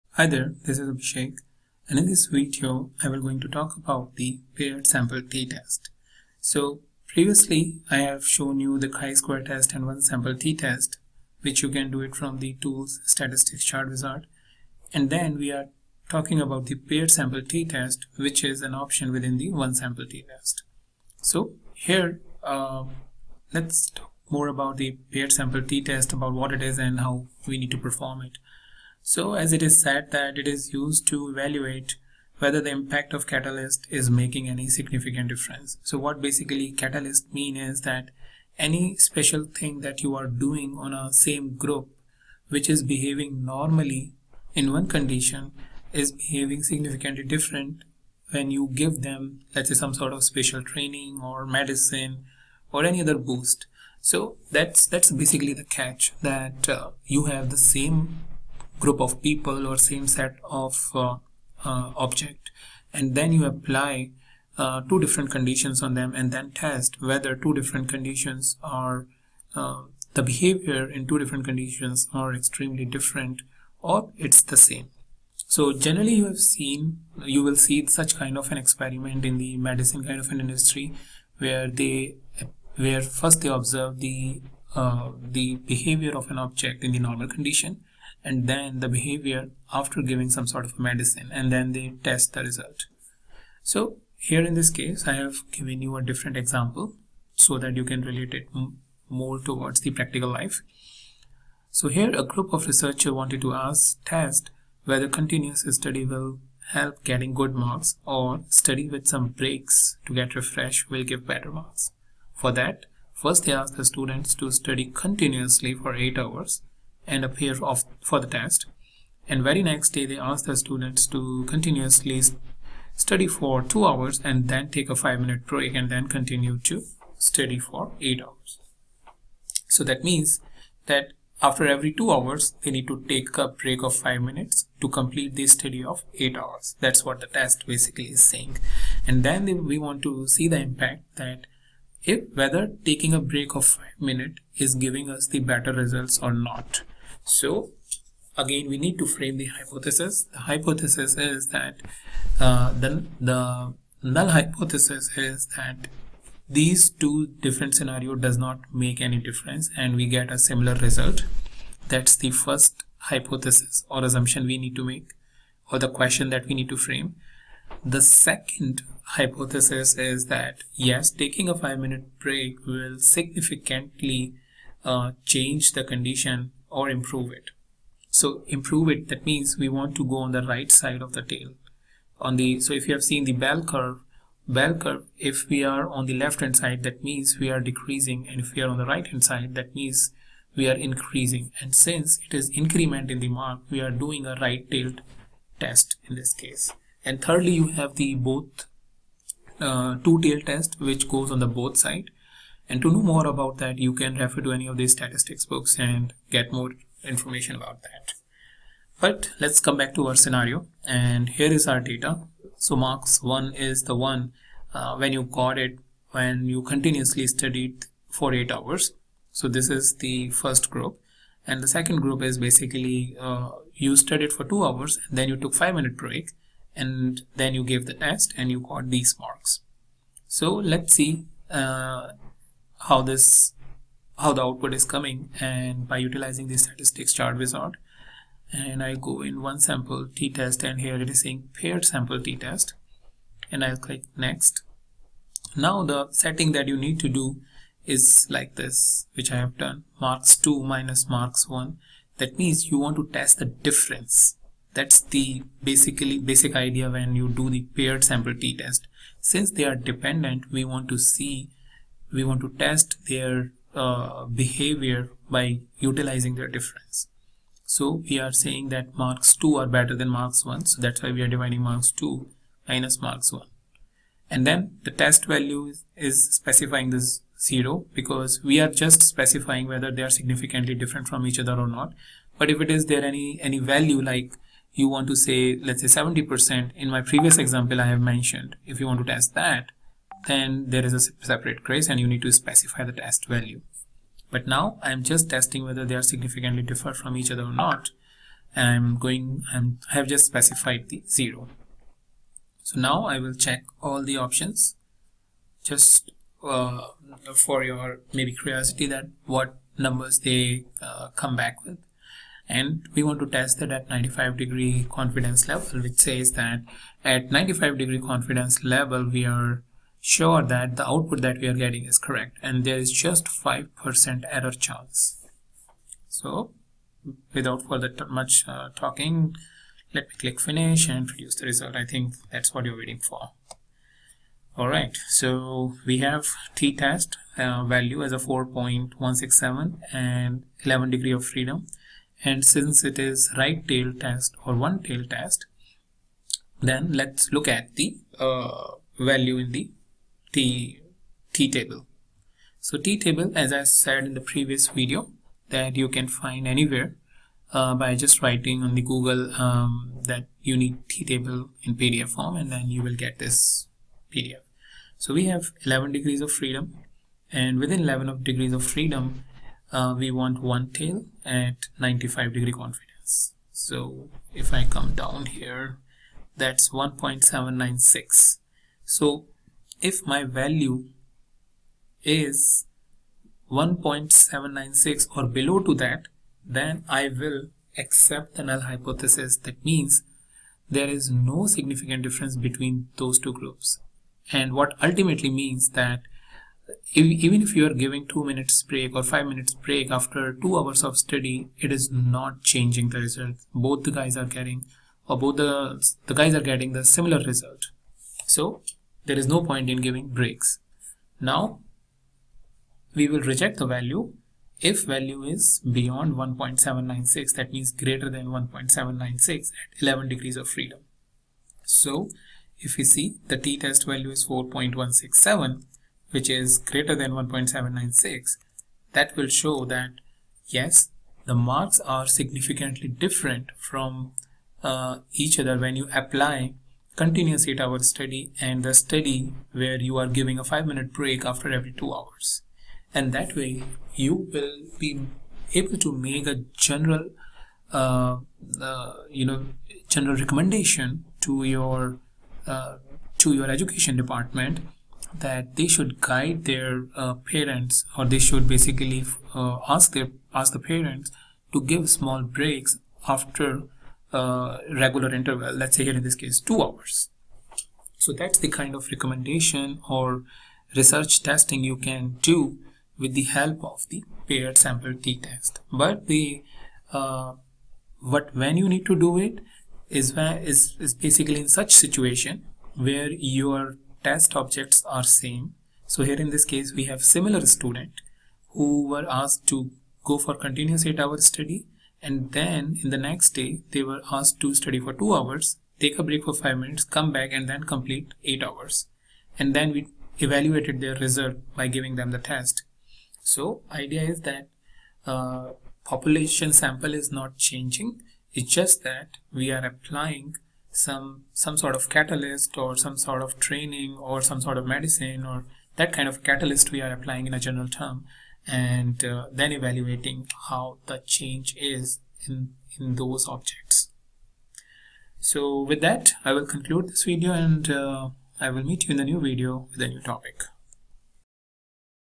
Qlikview Tutorial | Paired Samples T Test in QlikView | Data & Tools
Paired sample T Test is a commonly used method for researchers for evaluating the effectiveness of the medicine or special training or any other special treatment they are giving to similar set of users and then comparing the difference between their performance.

In this Data & Tools QlikView Tutorial video I've talked about how you can configure the statistical chart wizard to do the paired sample t test in qlikview.